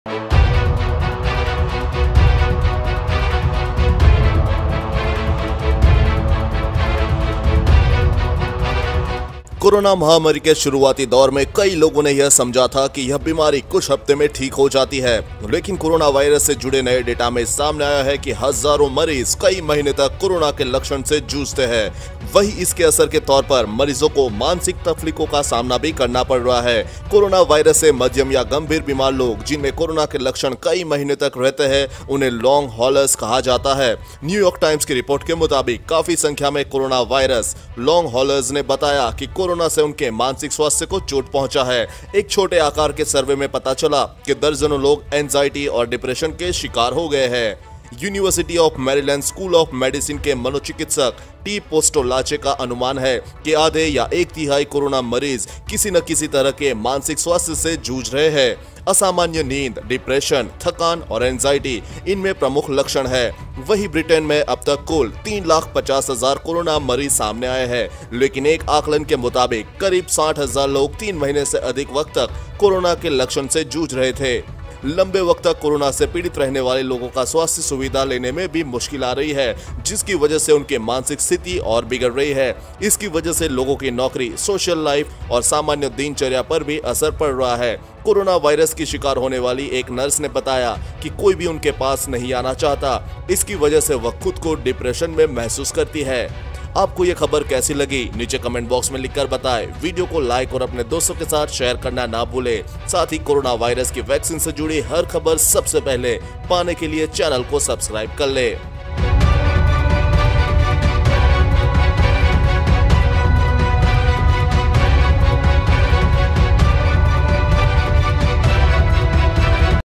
Corona Virus: कोरोना से ठीक होने के बाद इन बड़ी बीमारियों से झूझ रहे है मरीज
Corona Virus Latest News And Updates | Corona Virus Patients Are Suffering From These Serious Diseases After Recovering From Corona | Covid-19 Latest News And Updates

EDITOR ►Shadik